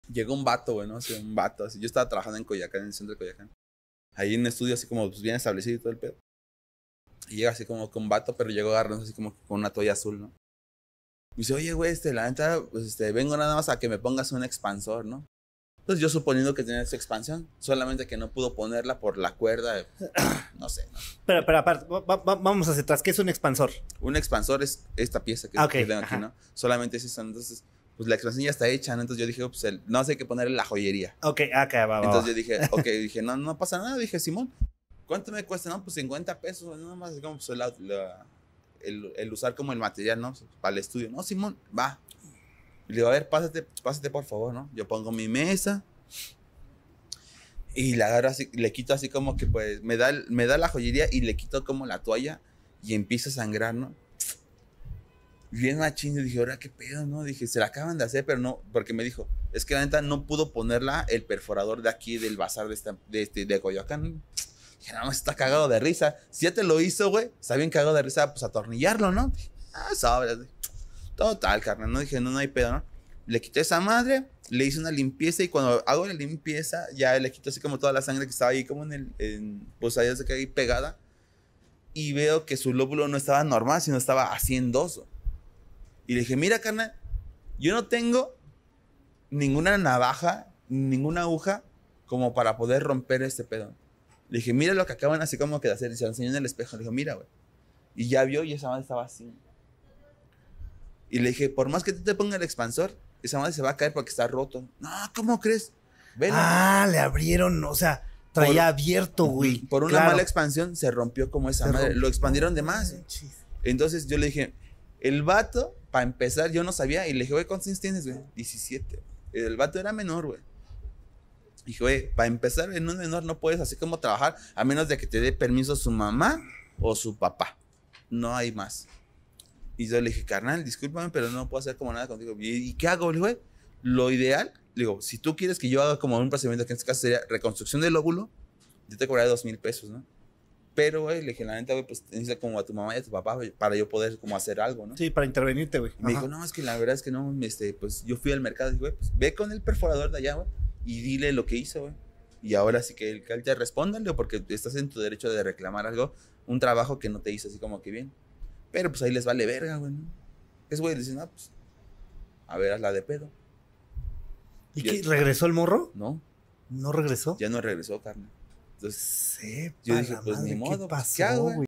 “Cuando una expansión SALE MAL” JUS PIERCER - ¿Implantes en el miembro? – Recordando el Podcast #48
Recordando cuando nos acompañó Jus Piercer en nuestro canal, modificador corporal profesional, quien nos explicó en aquella ocasión todo lo relacionado a la alteración permanente o parcial deliberada del cuerpo, así como algunas anécdotas de su experiencia.